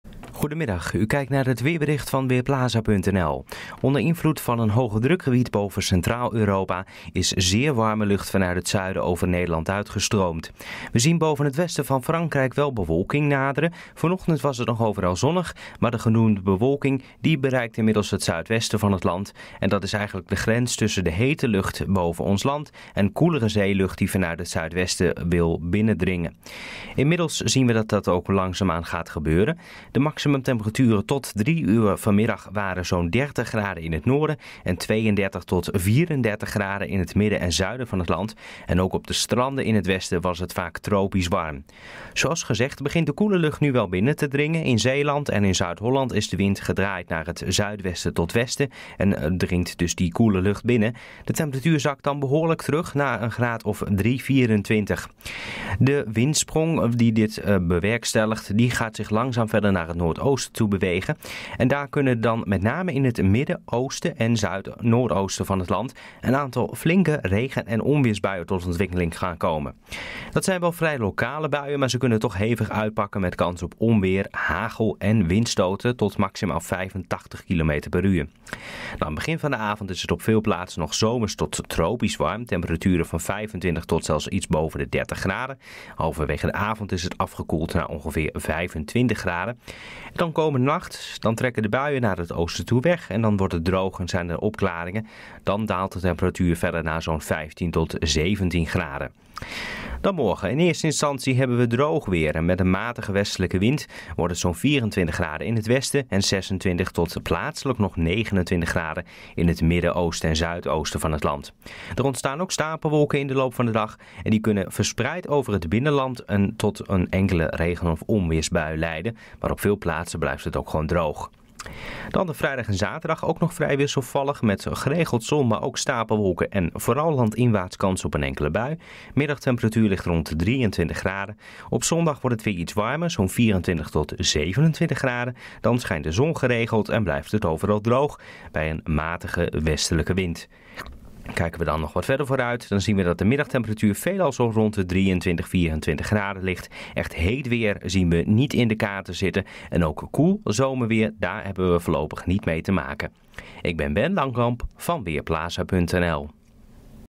Weerbericht woensdagmiddag: Lokale felle onweersbuien
Het is broeierig warm met temperaturen vaak boven de 30 graden. Vooral in het midden en oosten groeien forse stapelwolken lokaal uit tot stevige buien met onweer, en mogelijk ook hagel en (zware) windstoten. De wind is verder matig en draait geleidelijk naar zuidwest. Komende nacht wordt het geleidelijk overal droog en het kwik daalt tot rond 17 graden. Morgen is het minder heet met maxima van 23 tot 28 graden. De zon schijnt geregeld, maar landinwaarts valt er ook een enkele regen- of onweersbui.